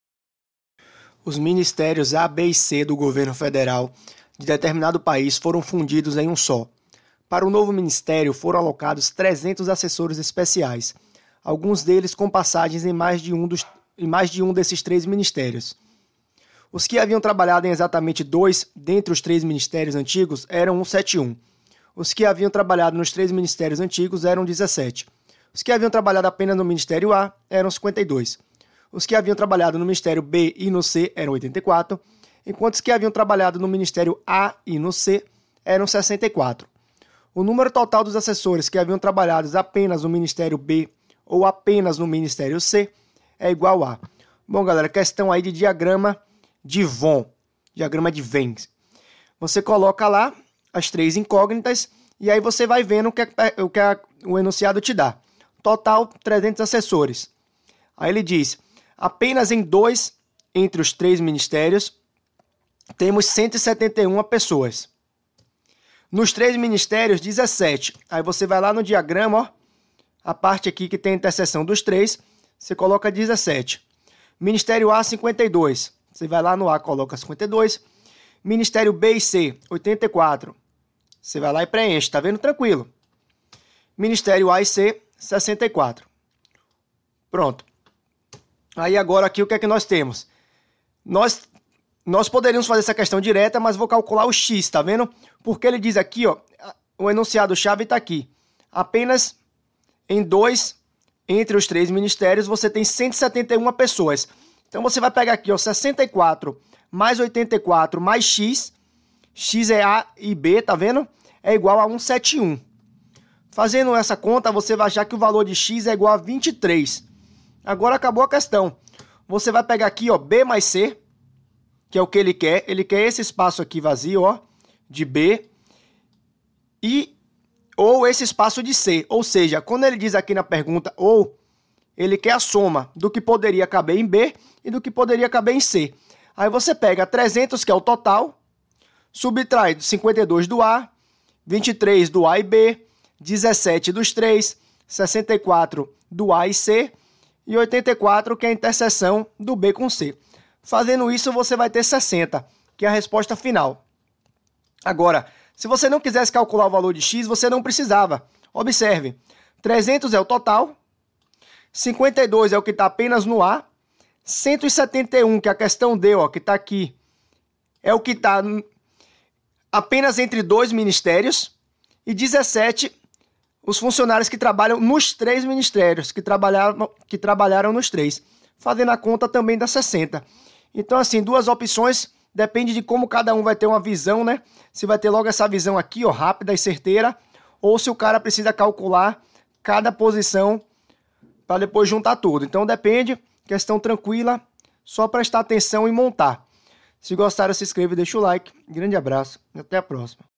FCC 2019 - Sefaz BA - Auditor Fiscal - Administração Tributária Q.23
Os ministérios A, B e C do Governo Federal de determinado país foram fundidos em um só. Para o novo ministério, foram alocados 300 assessores especiais, alguns deles com passagens em mais de um desses três ministérios. Os que haviam trabalhado em exatamente dois dentre os três ministérios antigos eram 171. Os que haviam trabalhado nos três ministérios antigos eram 17. Os que haviam trabalhado apenas no Ministério A eram 52. Os que haviam trabalhado no ministério B e no C eram 84, enquanto os que haviam trabalhado no ministério A e no C eram 64. O número total dos assessores que haviam trabalhado apenas no ministério B ou apenas no C é igual a
(A) 23.
(B) 60.
(D) 112.
(E) 141.